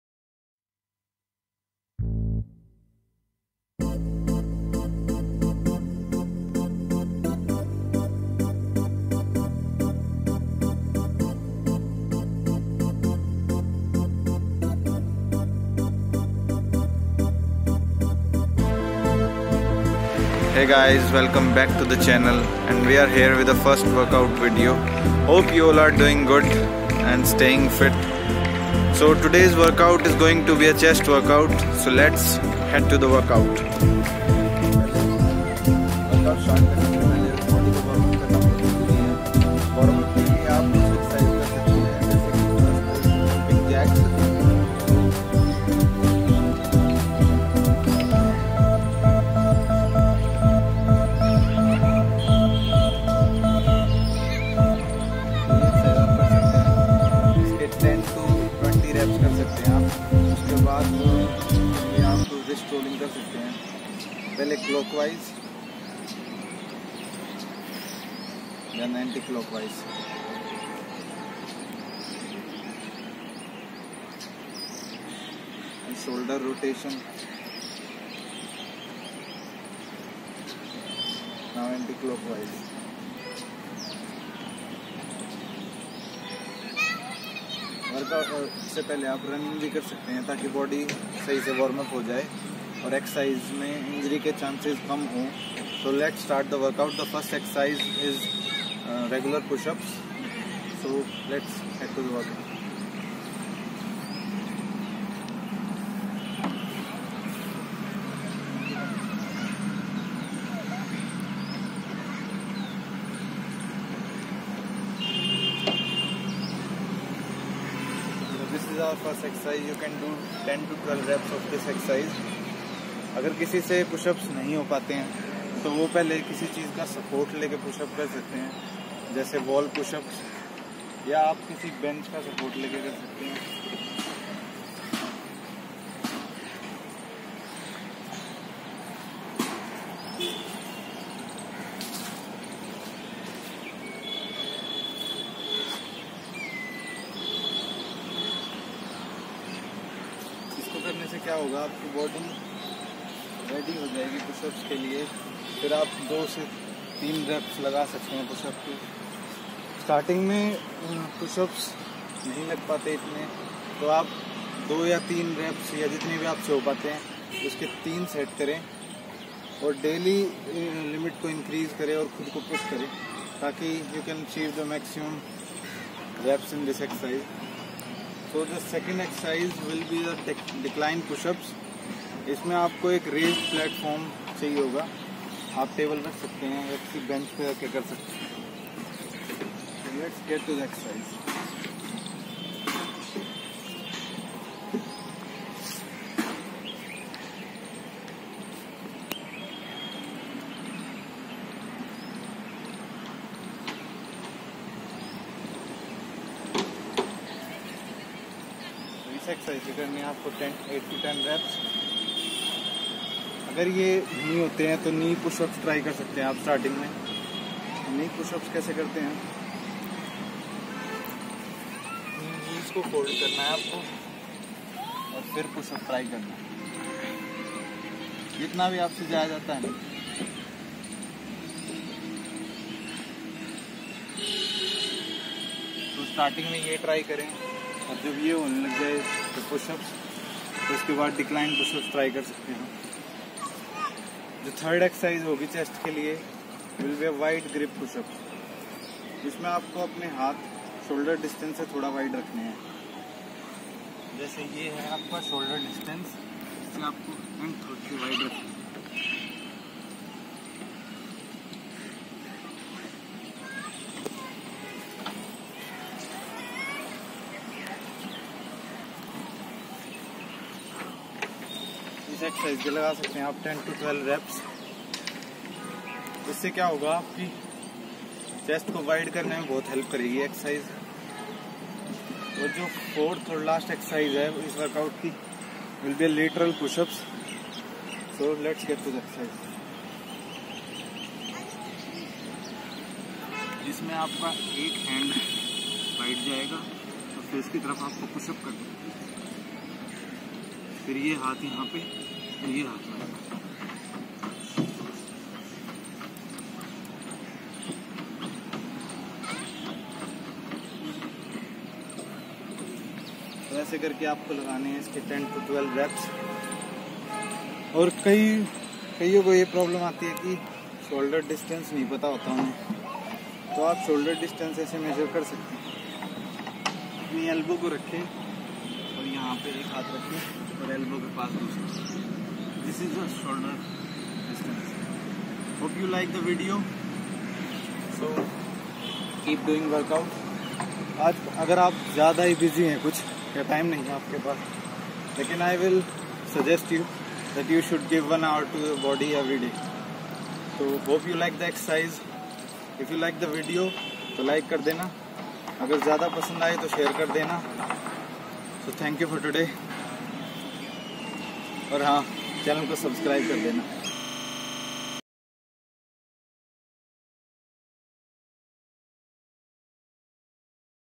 CHEST WORKOUT
WORKOUT DETAILS:
1. REGULAR PUSHUPS 10 - 12 REPS (3 sets)
2. DECLINE PUSHUPS 8 - 10 REPS (3 sets)
3. WIDE PUSHUPS 10 - 12 REPS (3 sets)
4. LATERAL PUSHUPS 10 -12 REPS (3 sets)